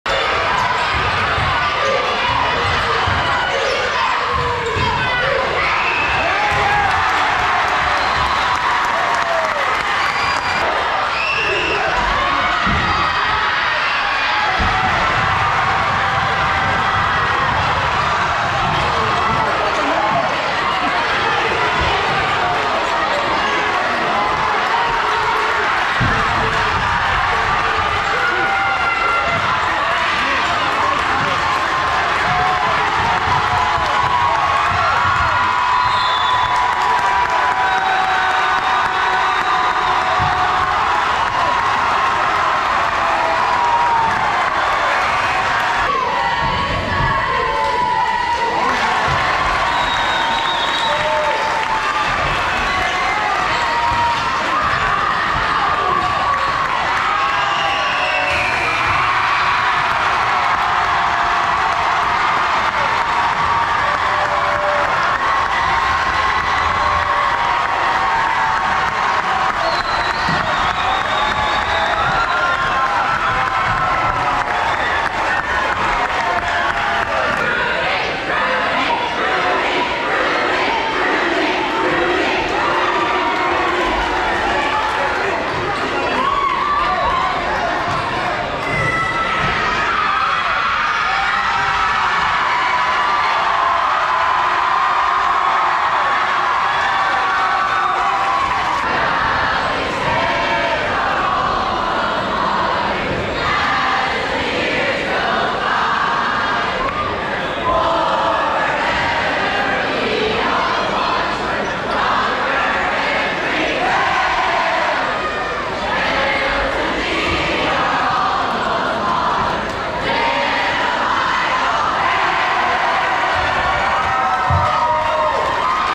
DSTV GoPro | Lady Jackets Advance To State Finals
Check out the view from our DSTV GoPro of a rocking and rowdy Hornsby Gym as the No. 2 Lady Jackets defeated Ruston 52-46 to punch their ticket to the State Finals in Hammond!